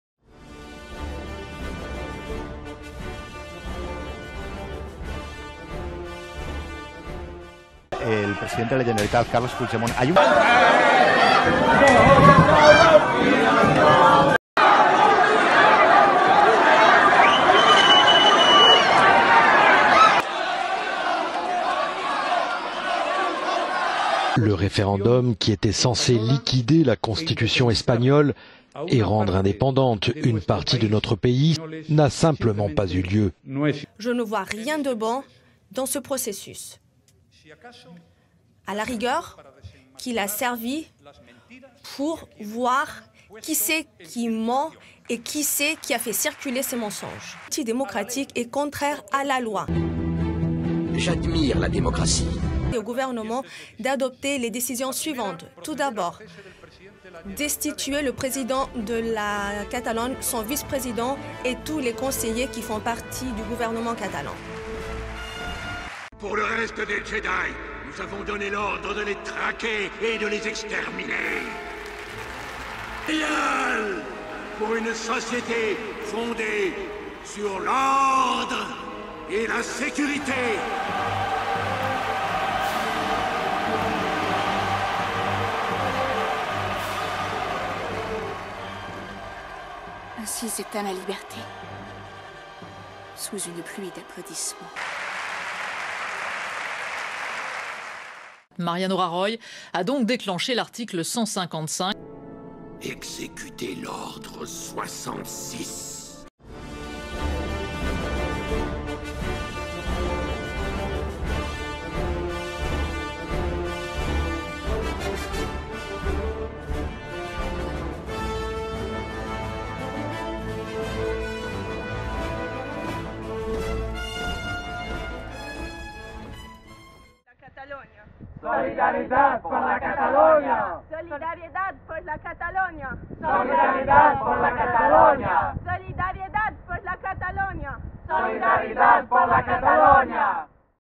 La fin de la démocratie en Catalogne
Malgré toute la tristesse que représente l'emprisonnement des représentants du peuple catalan et les atteintes à la démocratie, j'ai présenté la situation avec un brin de fantaisie. L'auteur du film dont j'emprunte des extraits a déjà déclaré qu' il s'inquiétait pour la démocratie dans son pays et que certaines scènes se voulaient un avertissement à ses concitoyens.